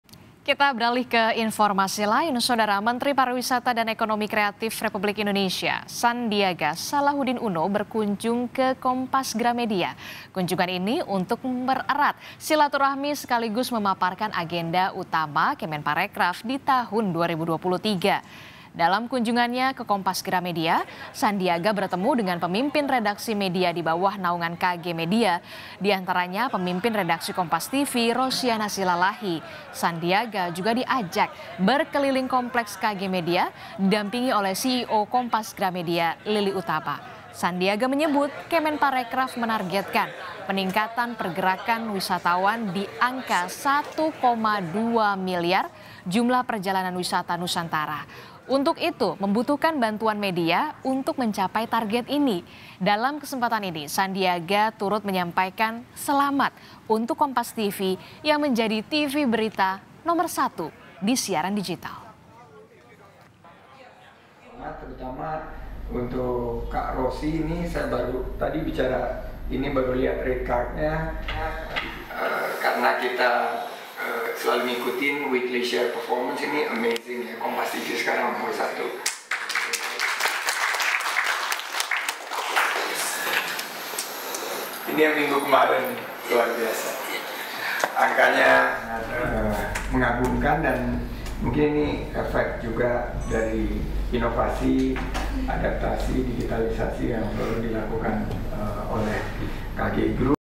Kunjungi Kompas Gramedia, Menteri PAREKRAF Sandiaga Uno: Luar Biasa Inovasi & Digitalisasi KG Media
JAKARTA, KOMPAS.TV - Sandiaga Uno selaku Menteri Pariwisata dan Ekonomi Kreatif (PAREKRAF) RI, bertemu dengan Pemimpin Redaksi Media di bawah naungan KG Media, di antaranya Pemimpin Redaksi Kompas TV, Rosianna Silalahi.

Sandiaga juga diajak berkeliling Kompleks KG Media, didampingi oleh CEO Kompas Gramedia, Lilik Oetama.

Sandiaga menyebut, Kemenkarekraf menargetkan peningkatan pergerakan wisatawan di angka 1,2 miliar jumlah perjalanan wisatawan Nusantara.

Untuk itu, membutuhkan bantuan media untuk mencapai target ini.

Dalam kesempatan ini, Sandiaga turut menyampaikan selamat untuk Kompas TV yang menjadi stasiun televisi berita nomor satu di siaran digital.